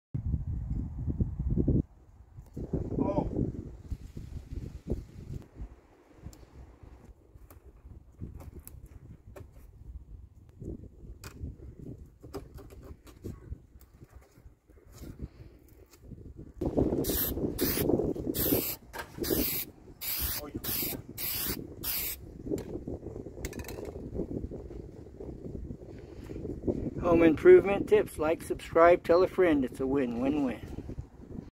Painting Screw Heads
I will show you how to, Quickly Paint Screw Heads,  to any Color.
...   Spray painting screws can be a frustrating thing to do.
   In this video,  I will show you a Quick, and easy way, to  Spray Paint Screwheads.

.KensHowTos HomeImprovmentTips b#shortvideos .